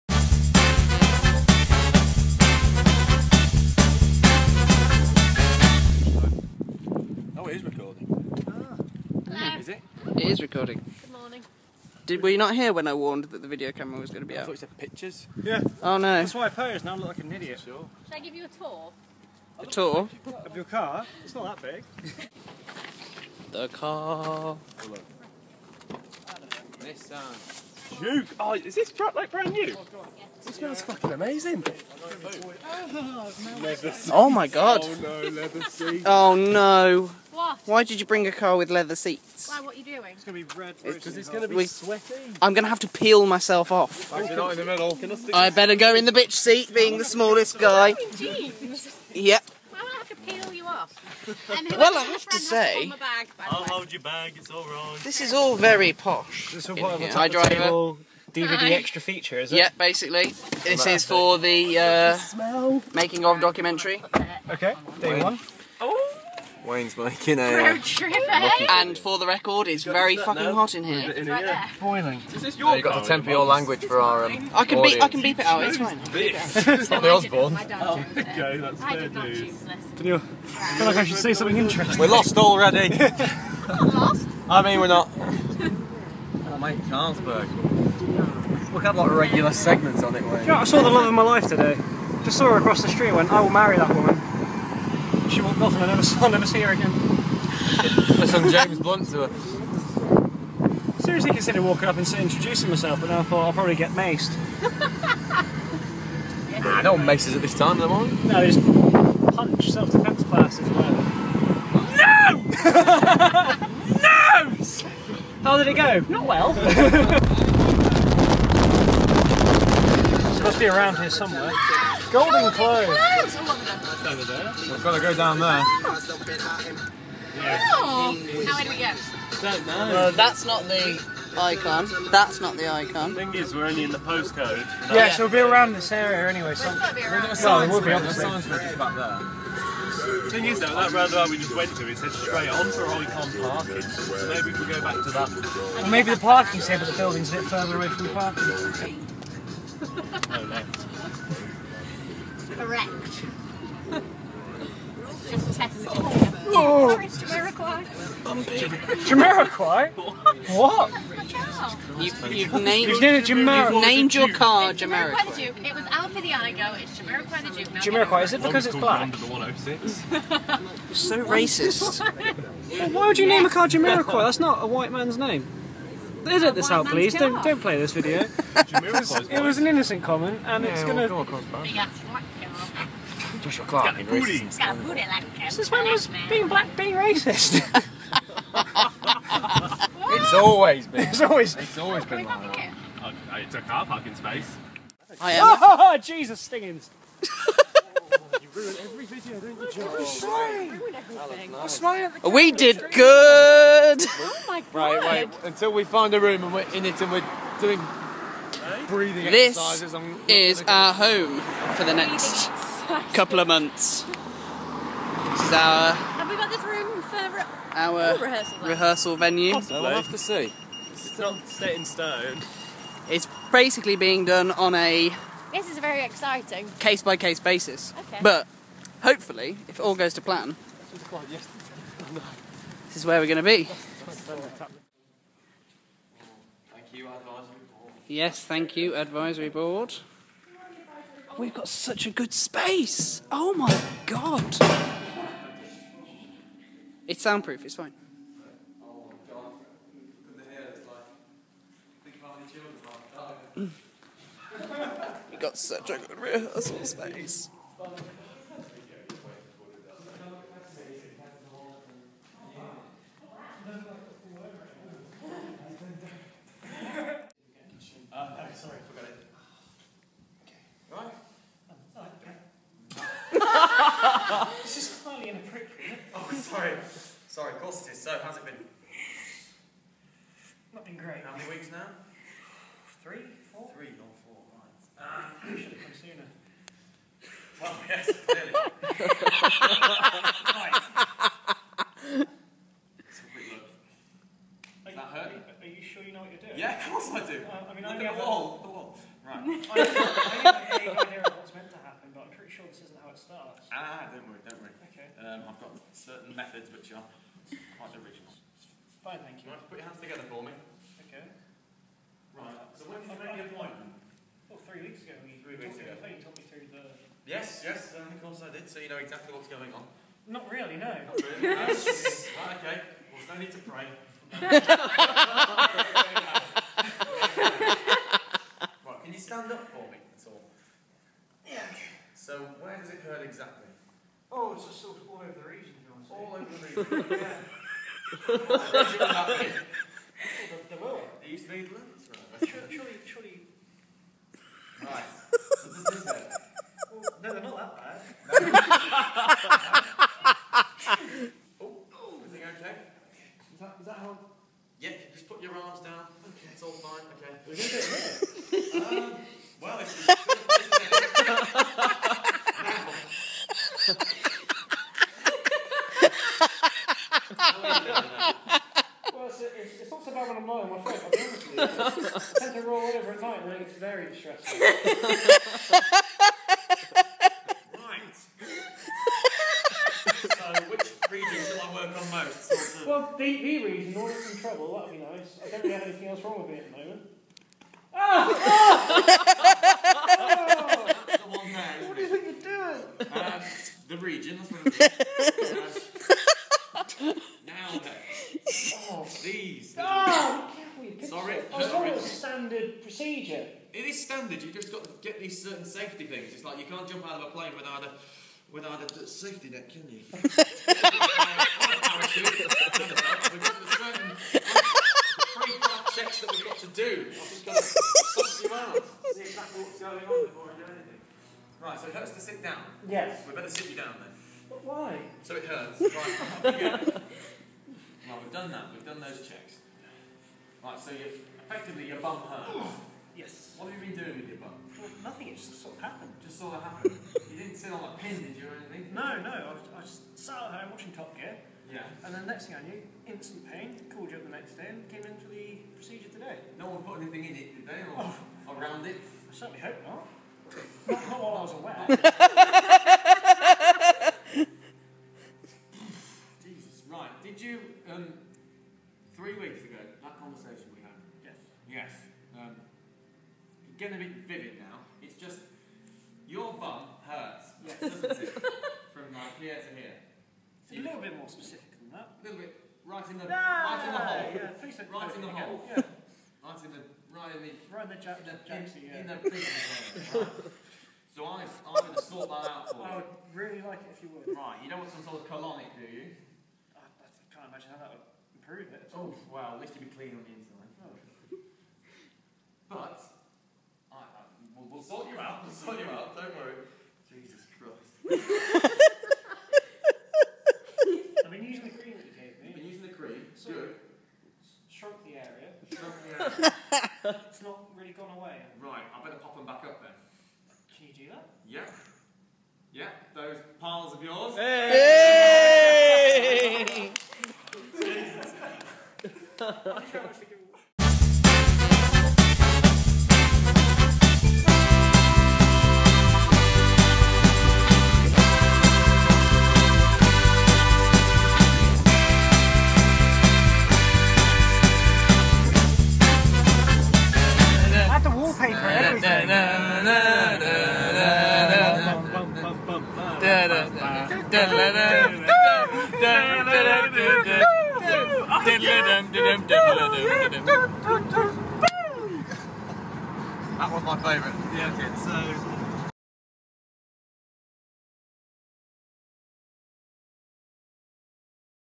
Gasping Behind the Scenes Part 1
Observe Tap the Table Theatre as they prep for their second rehearsal of Ben Elton's 'Gasping'

Note: Contains Adult Language